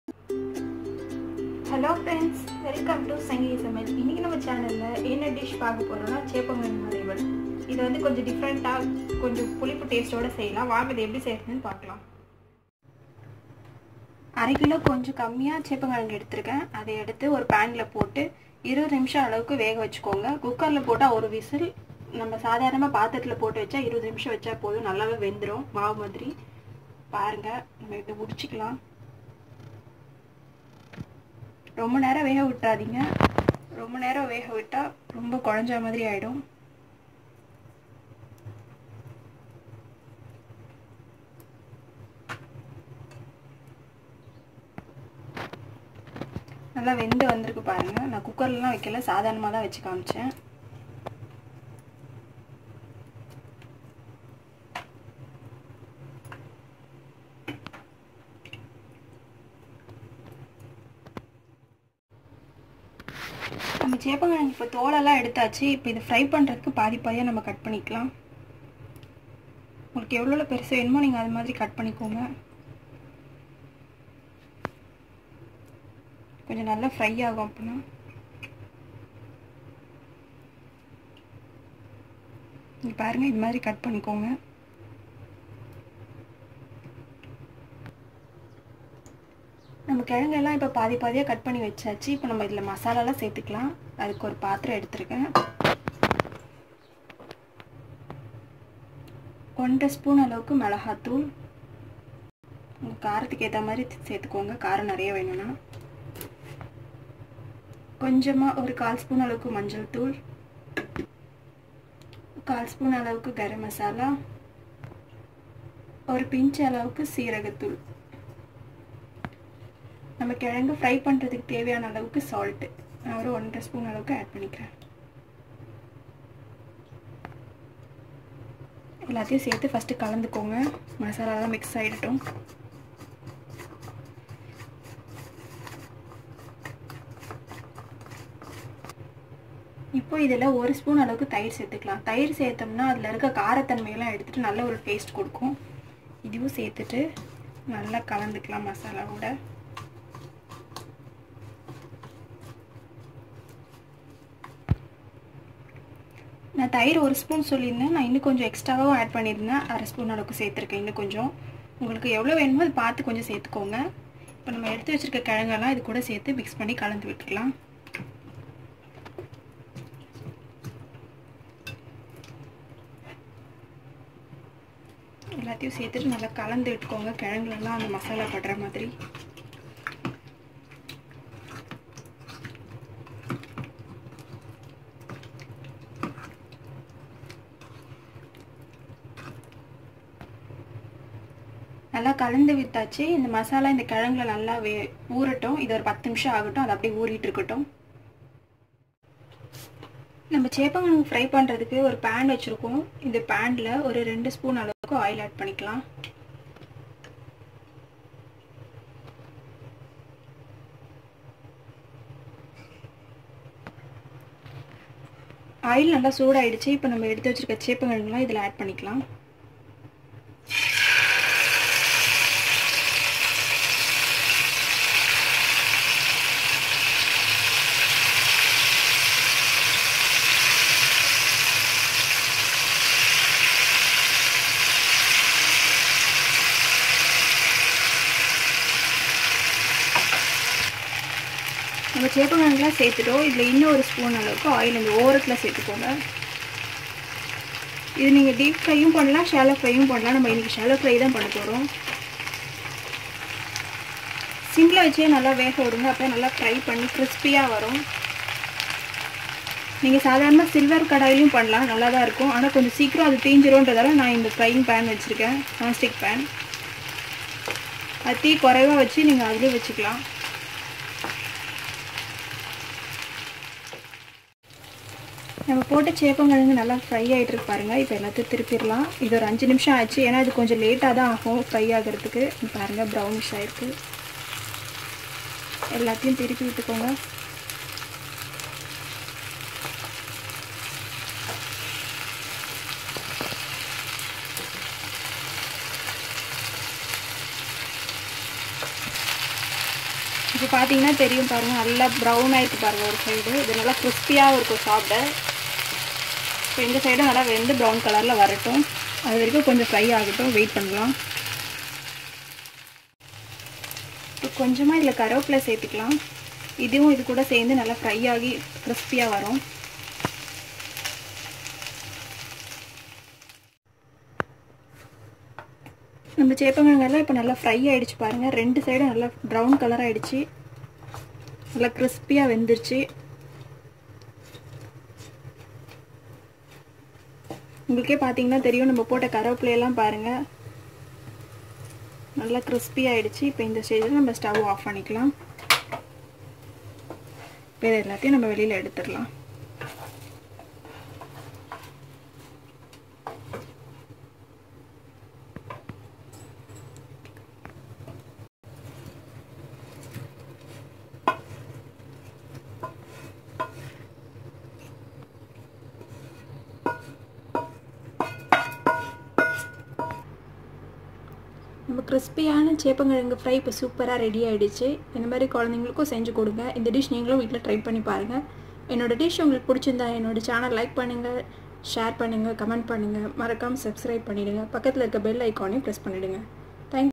Sepankizhangu fry in tamil |Seppankizhangu Roast - Seppankizhangu Fry- Arbi (Taro root) Fry Recipe
Arbi or Taro roots are rich with carbohydrates, vitamins, minerals, fibre. It is a complete food with rich nutrition value added to it. It is good for digestive system, blood pressure, diabetes, skin health, eye vision, and prevents cancer as well. Though roasting is not the most healthy way to consume it, it is one of the yummiest ways to have it. It goes well with all kinds of rice varieties. I personally love with with curd rice. I am sure you will have your own favourite combination.

Ingredients

250 grams Seppankizhangu (Arbi) or Taro root
2 tbsp chilli powder
half tbsp turmeric
half tsp garam masala
pinch of cumin powder
curd as you required
Salt to taste
Oil to cook